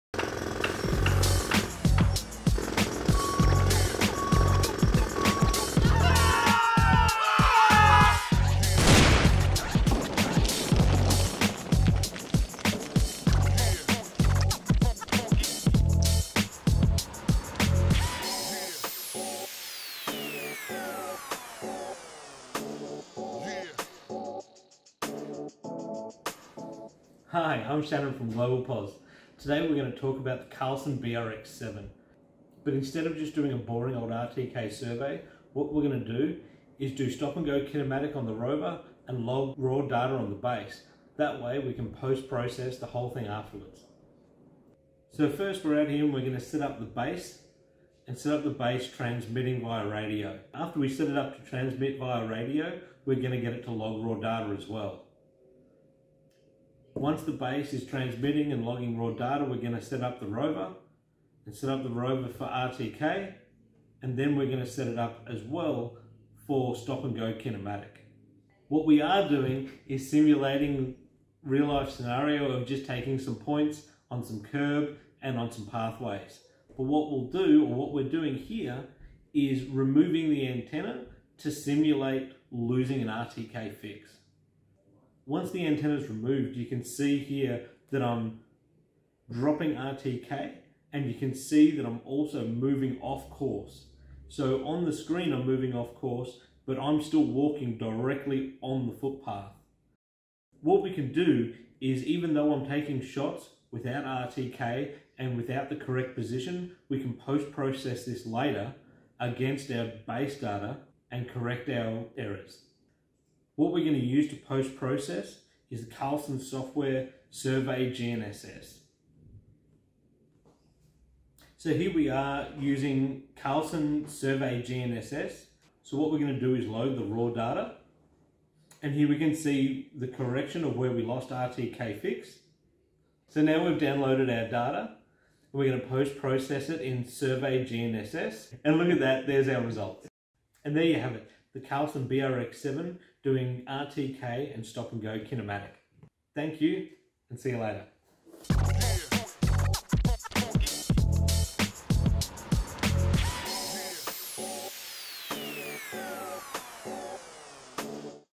Carlson BRX7 review with stop and go kinematic
Today we review the BRX7 with its stop and go kinematic funtions.
for more info!